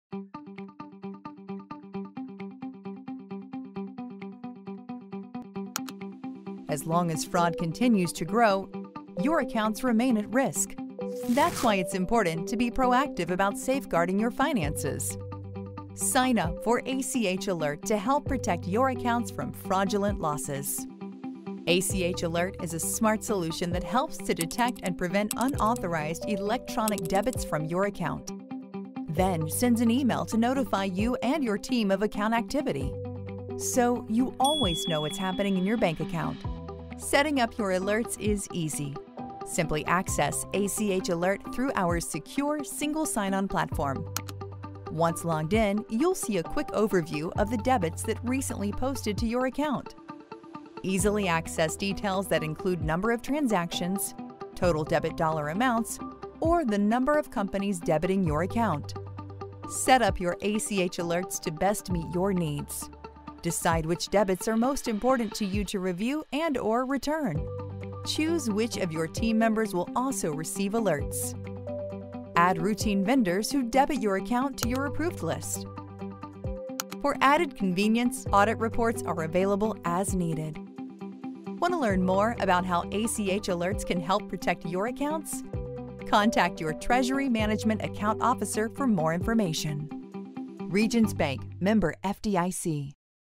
How to Set Up Alerts for ACH Accounts
Protect your accounts and safeguard your finances from fraud with ACH Alert. It's easy to set up alerts - watch and learn now.